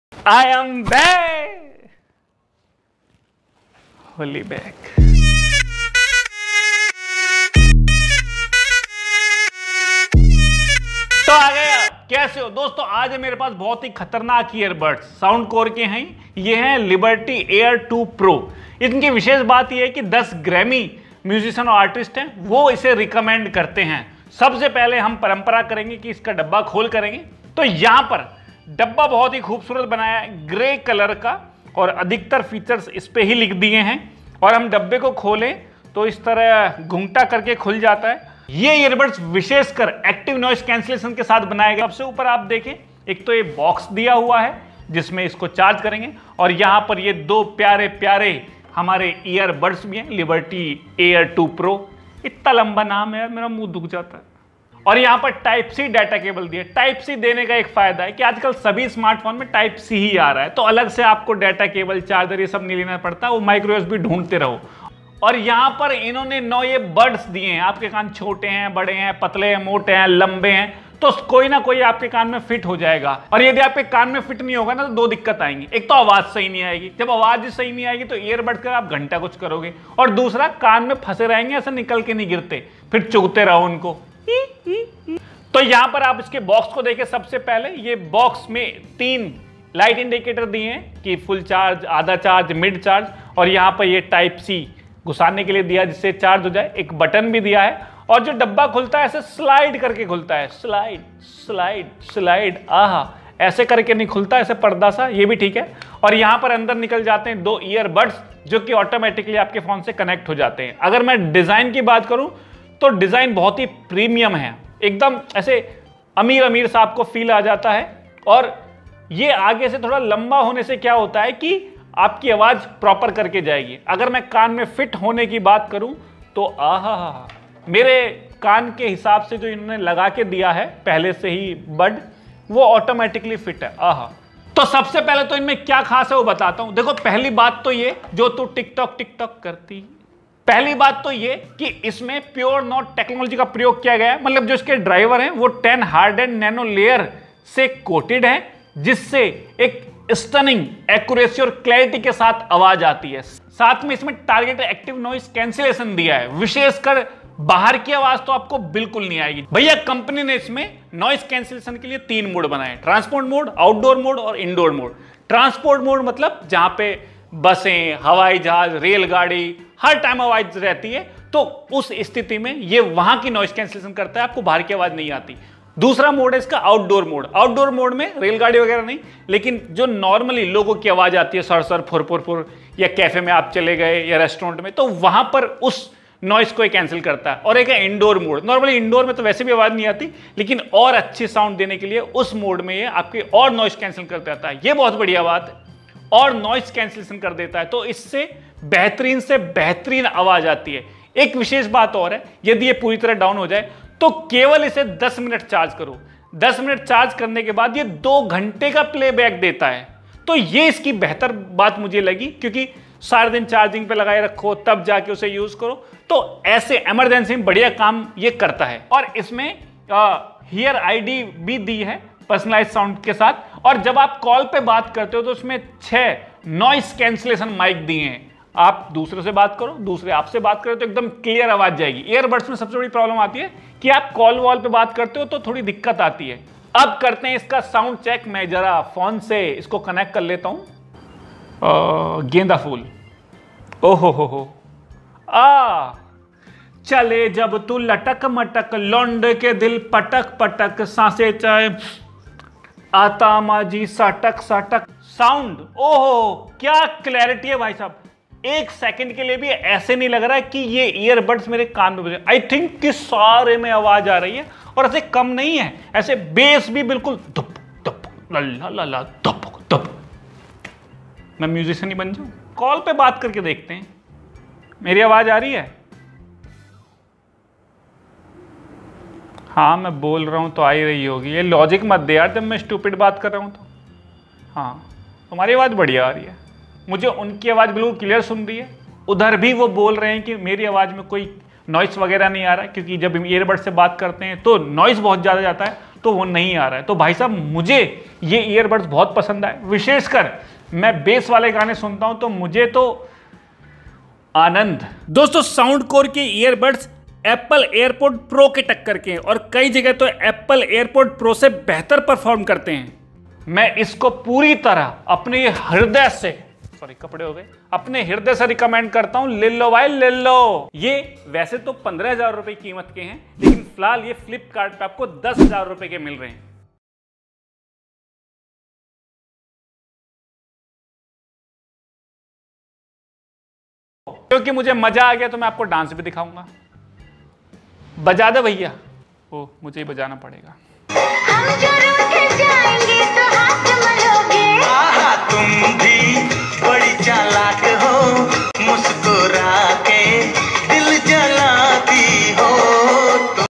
Kaan Faad Earbuds | Soundcore Liberty Air 2 Pro
My Dear Technical Dost Family
Is Video me Technical Dost btaya hai Soundcore Truly Earbuds
My Setup
Camera (Samsung Note 8)
Mic (Boya M1)